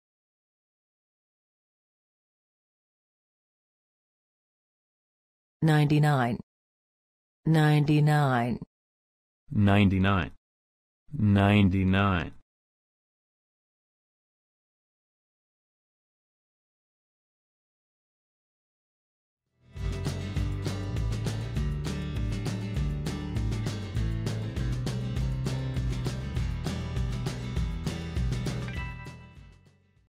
99 इंग्लिश में क्या है
99 को अंग्रेजी में सही तरह से कैसे बोलें। ध्यान से सुनो।
99 का हिंदी से अंग्रेजी में सही अनुवाद क्या है?आप अमेरिकी व्यक्ति की यरह बोलिए।